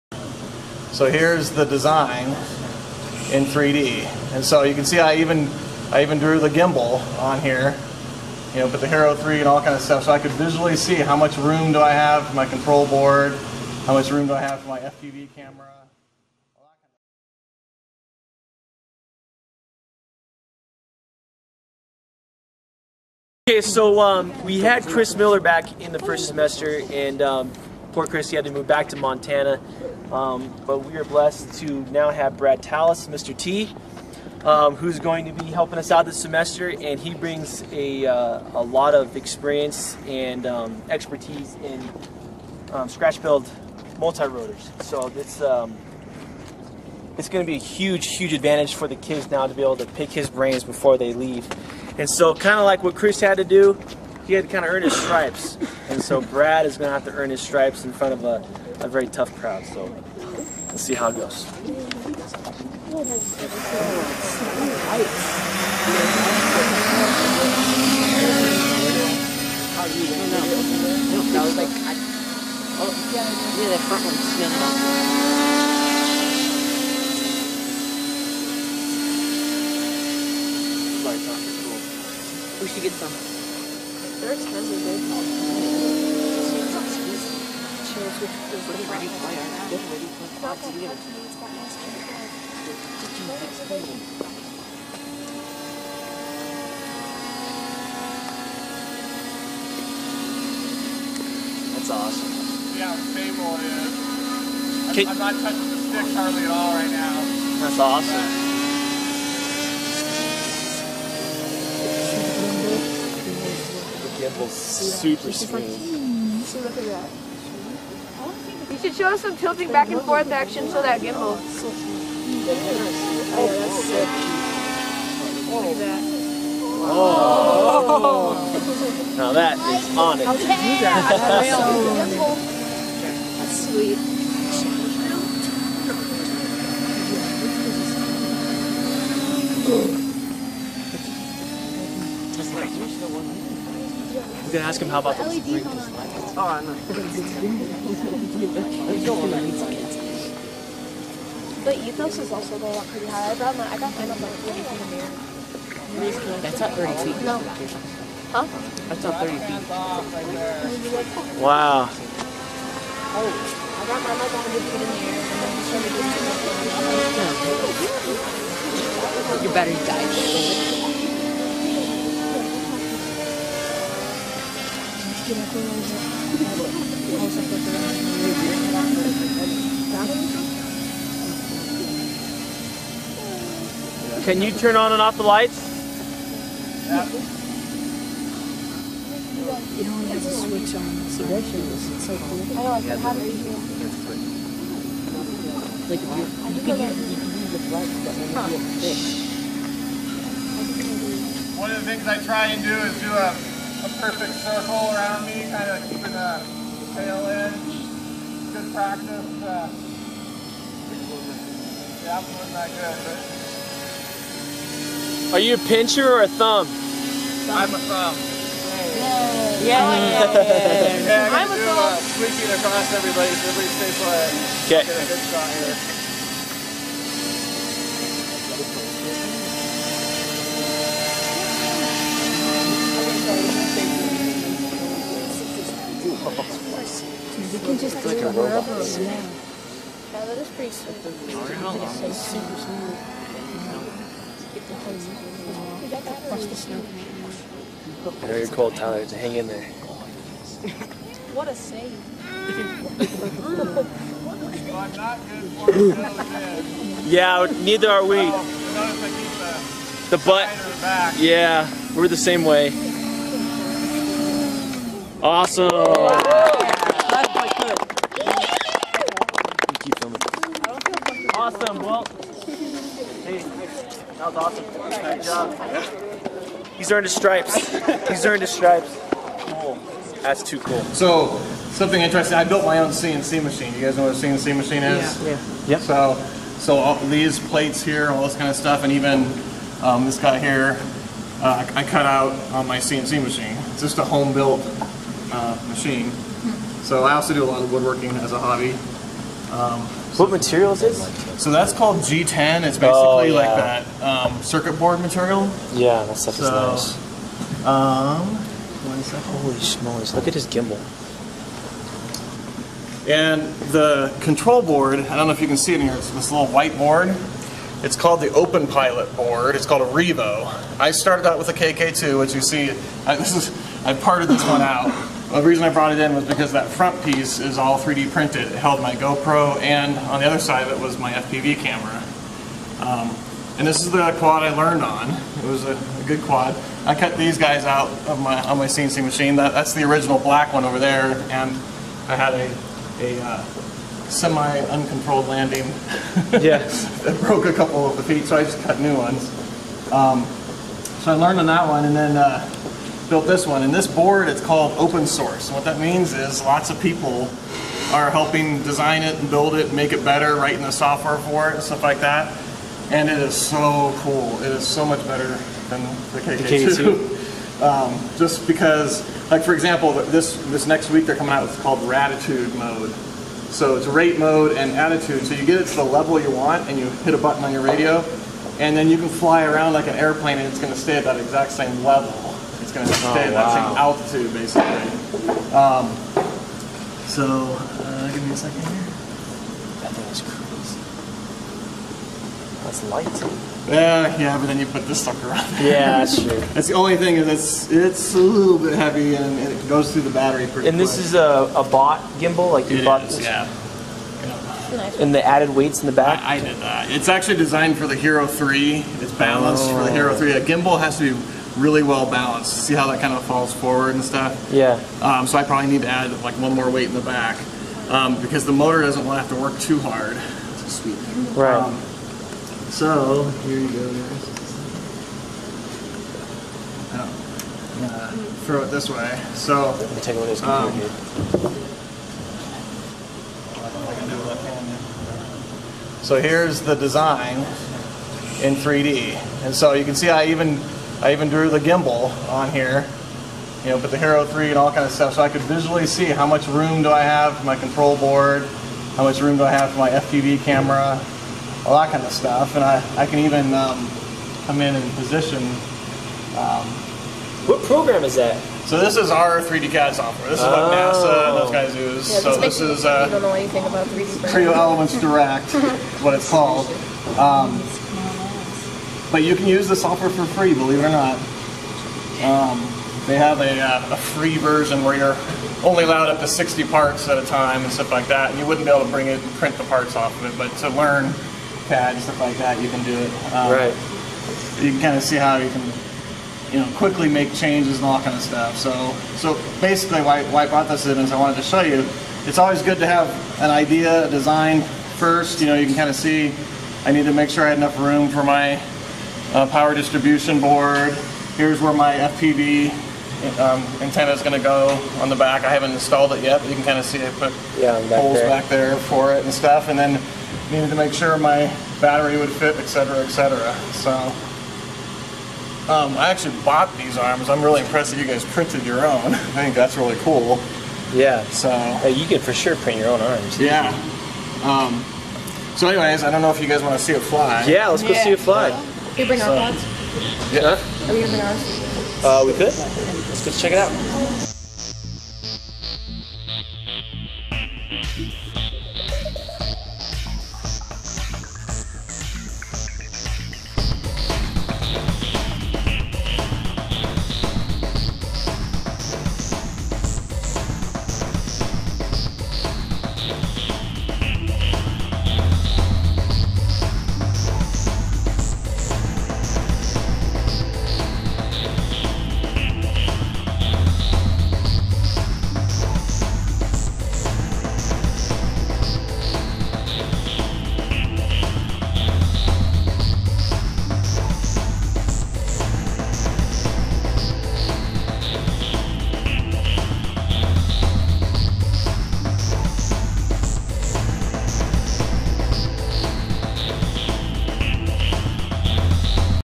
MESArcFF - CNC Quadcopter - VOLUNTEER
EPS 23 S4,
This episode features Brad Tallis, MESArcFF newest volunteer.  A 3D CAD software engineer by day and a scratch build multicopter creator by night.  Huge score for the MESA students.

For more information about Mr. T, please visit our article on FliteTest

MESArcFF Team